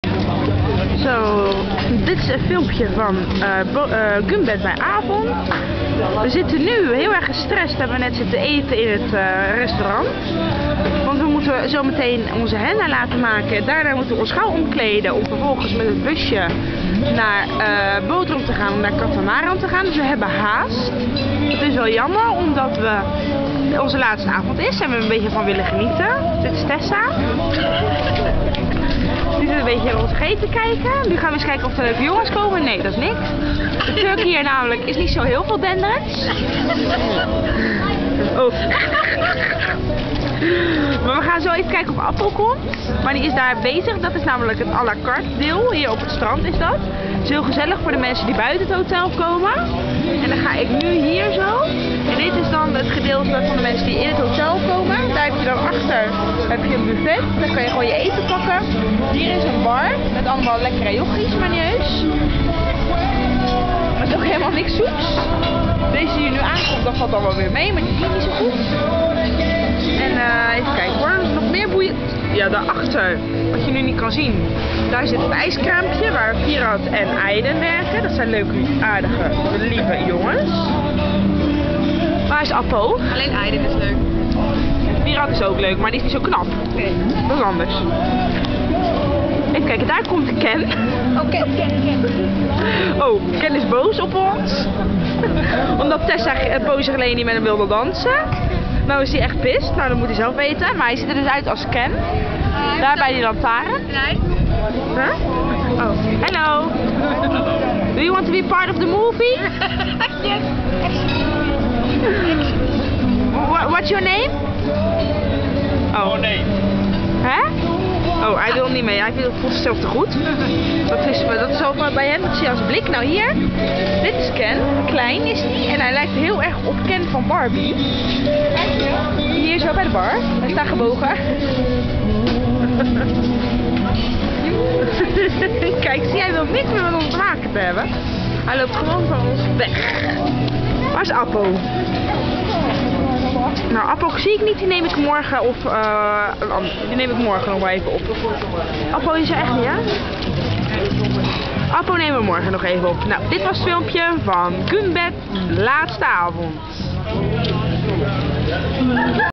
Gümbet 2008 Dinner Sami Beach
Anne talks about the people on the boulevard..